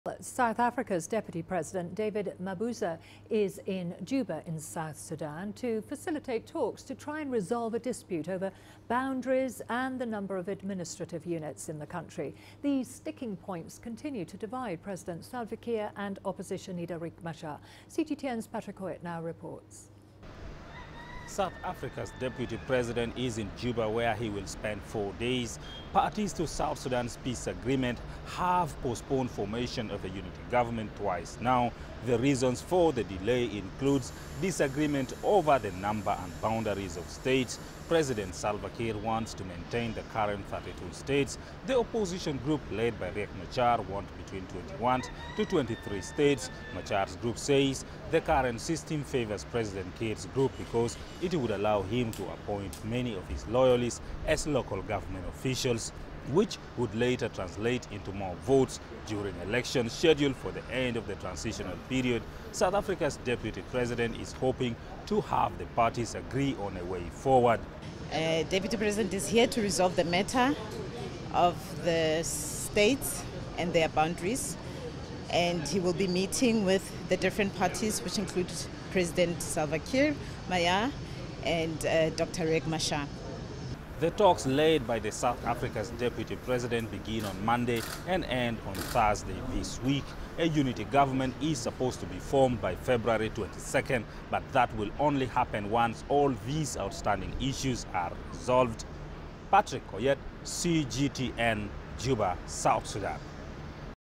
South African deputy president in Juba to facilitate talks
South Africa's Deputy President, David Mabuza, is in Juba, South Sudan, to facilitate talks to try and resolve a dispute over boundaries and the number of administrative units in the country.  These sticking points continues to divide President Salva Kiir and opposition leader, Riek Machar. CGTN's Patrick Oyet reports.